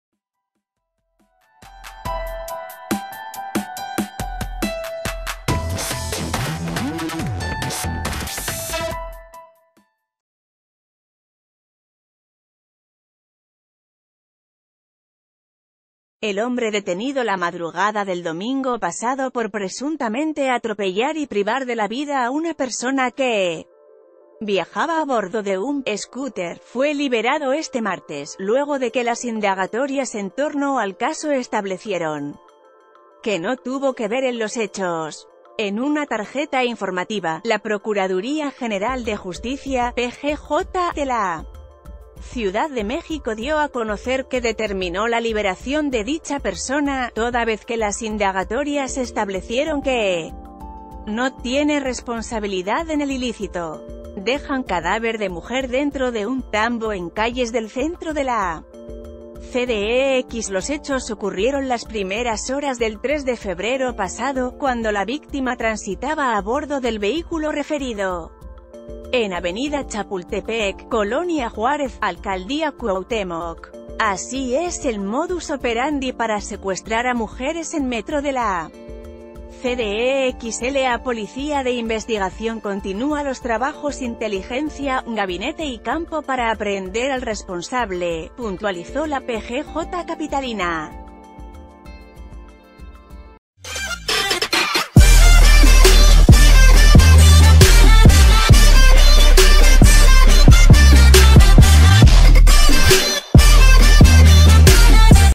Liberan a taxista detenido por muerte de usuario de scooter
El hombre detenido la madrugada del domingo pasado por presuntamente atropellar y privar de la vida a una persona que viajaba a bordo de un "scooter", fue liberado este martes, luego de que las indagatorias en torno al caso establecieron que no tuvo qué ver en los hechos. En una tarjeta informativa,...